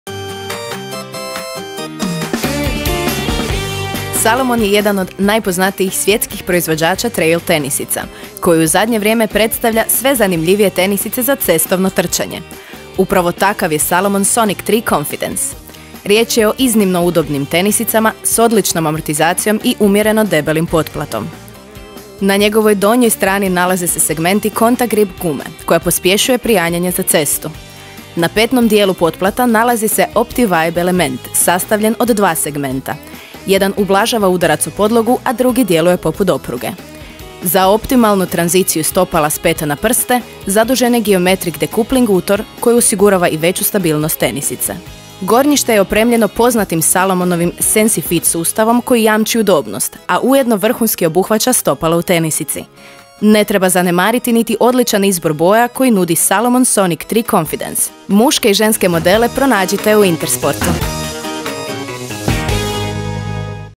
Salomon Sonic 3 Confidence
Salomon je jedan od najpoznatijih svjetskih proizvođača trail tenisica, koji u zadnje vrijeme predstavlja sve zanimljivije tenisice za cestovno trčanje. Upravo takav je Salomon Sonic 3 Confidence.